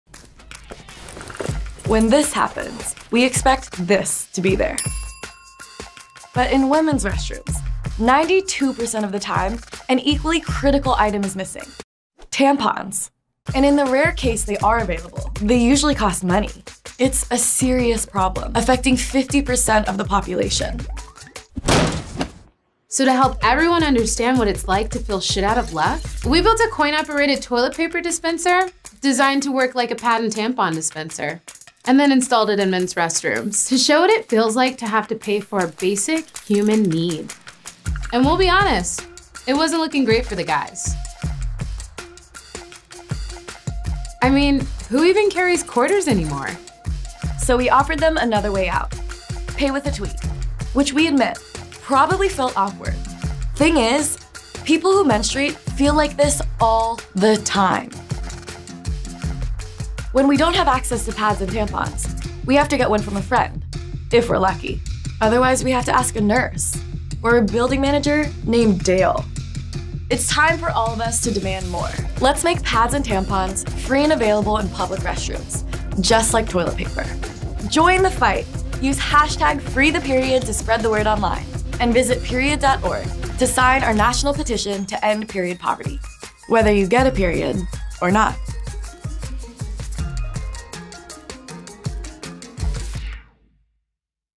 PERIOD's Toilet Paper Dispenser
PERIOD marked International Women's Day 2020 by imagining what it would be like if men had to pay for toilet paper the way women have to pay for menstrual products. The result: a coin-operated toilet paper dispenser.